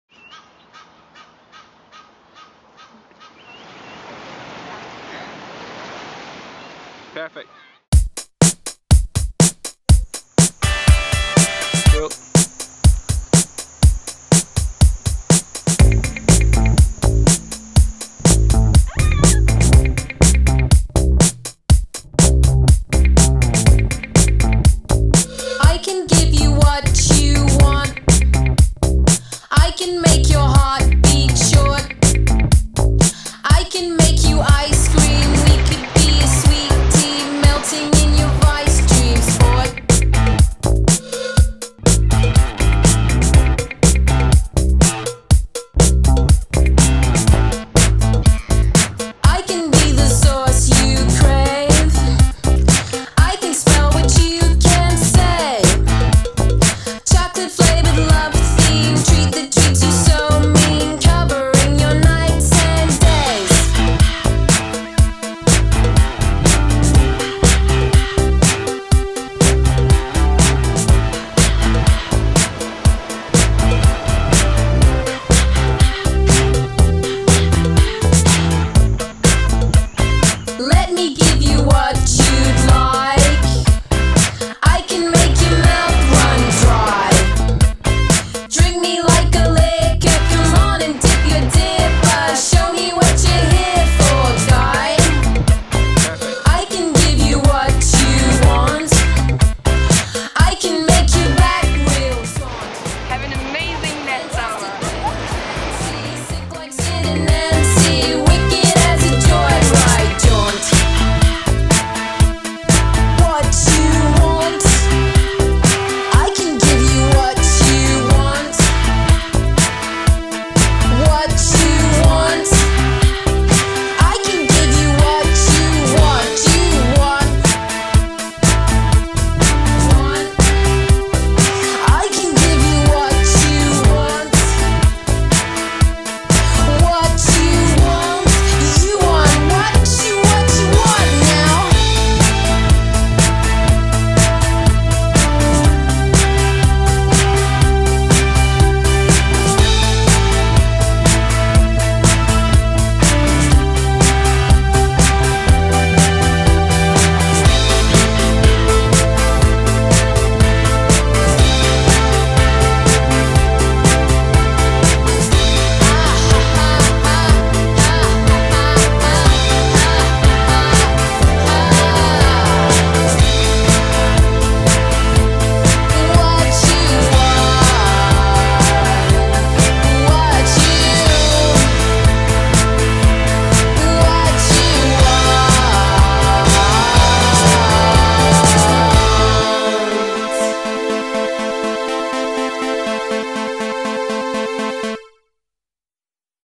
Ness Photo Shoot
Here's a little film of out photo shoot in Cape Town this January.